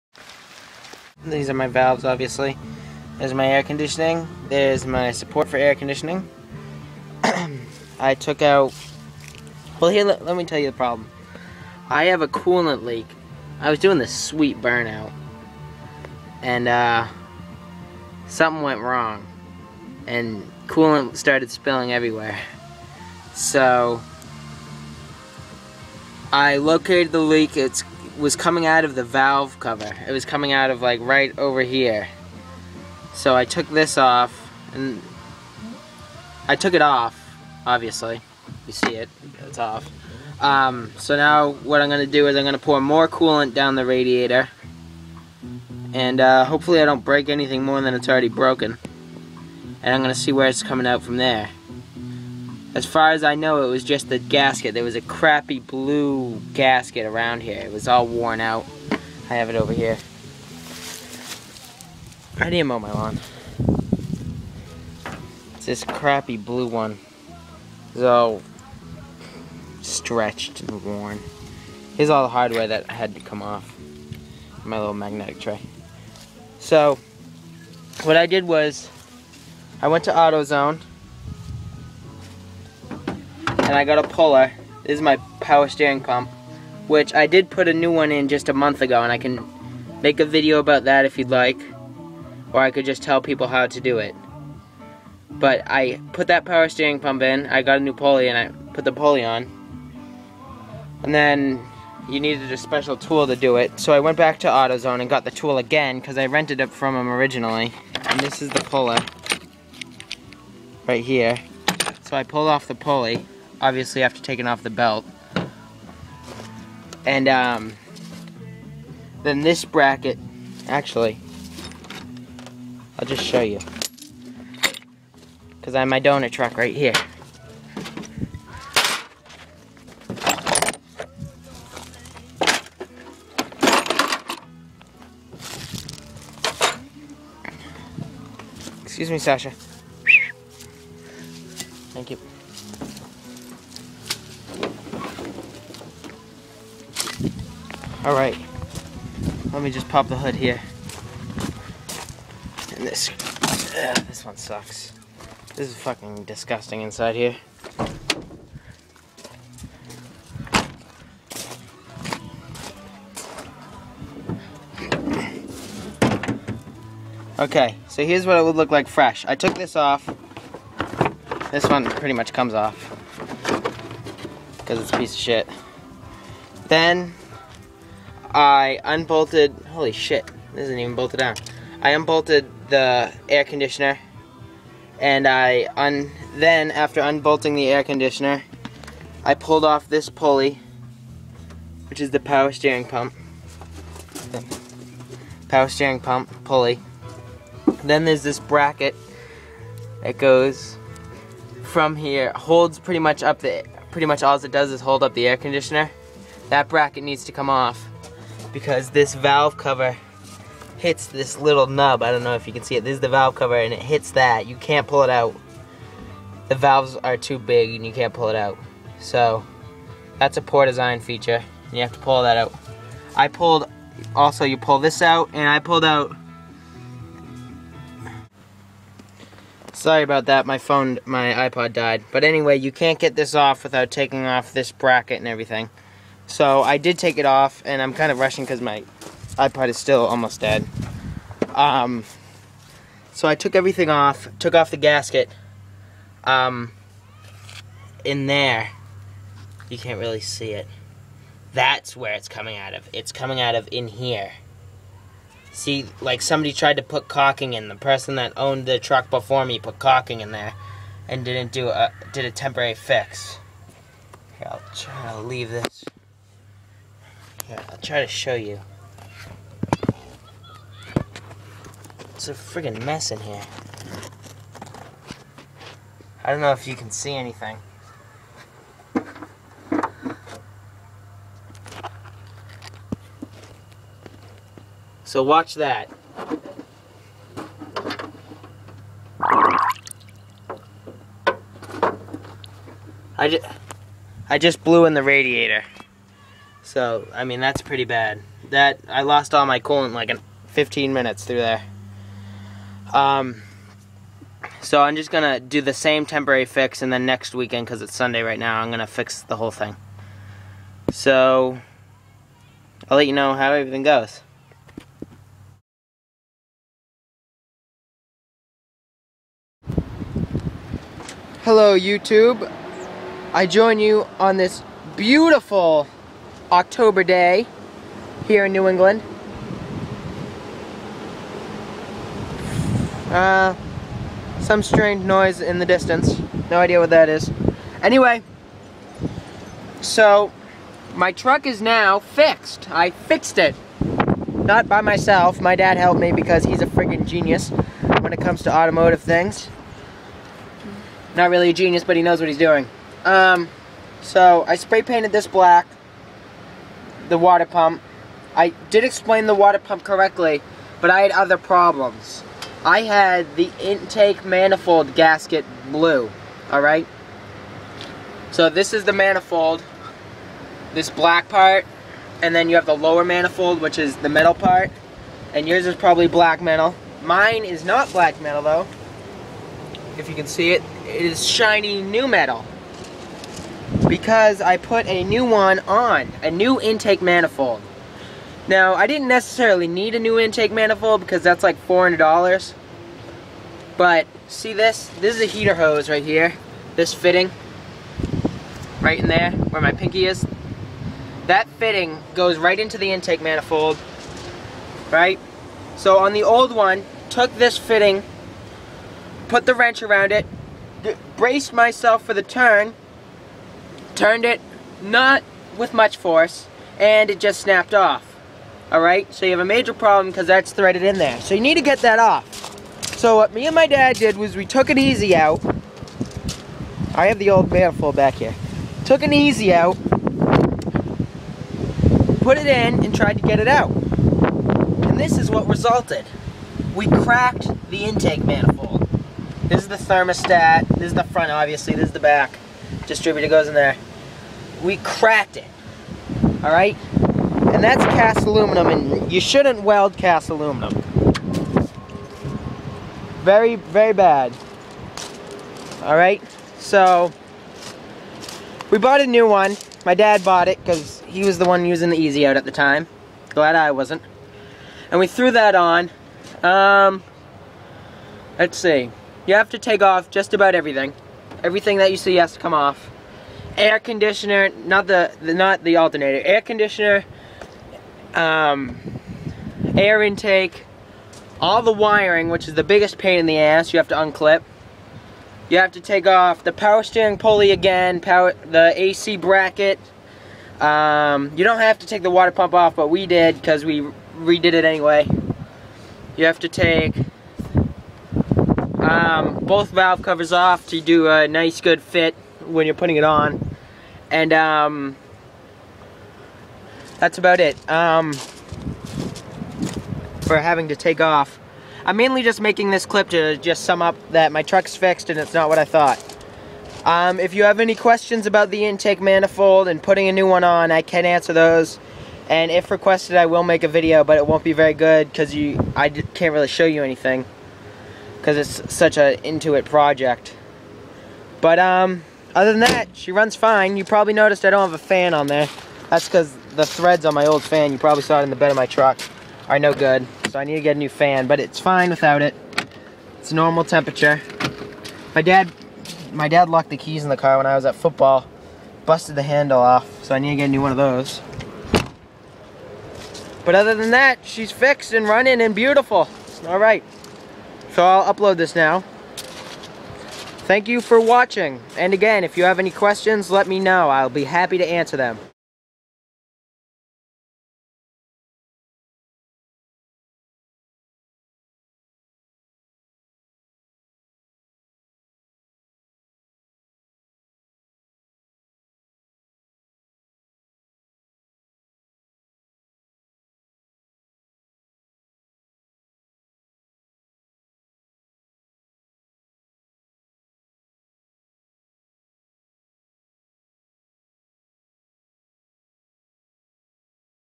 Coolant Leak Diagnostic and Repair Pt. 2
*(I don't know much about engines so I may seem like a moron most of the time for thinking coolant was in the valves but this is how you learn, right!)* I had a coolant leak in my truck. I thought it was the water pump, then the valves figured out it was the Intake manifold Gasket. I also did my timing chain and gears if you would like to know how to do that.